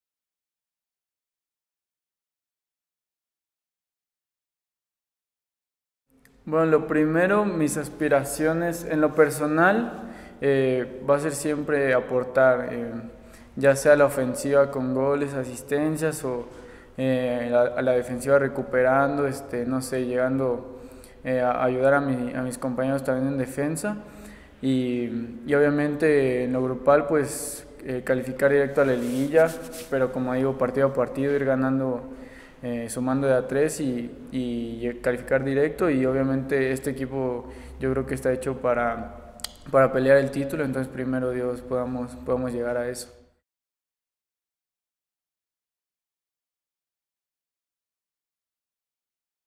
Conferencia de Prensa | Puedo aportar mucho al equipo, trabajaré para cumplir mis objetivos🎽
Nuestro Poblano habló desde el Cuauhtémoc ¡Checa todo lo que dijo Iván Moreno después de su debut con La Franja!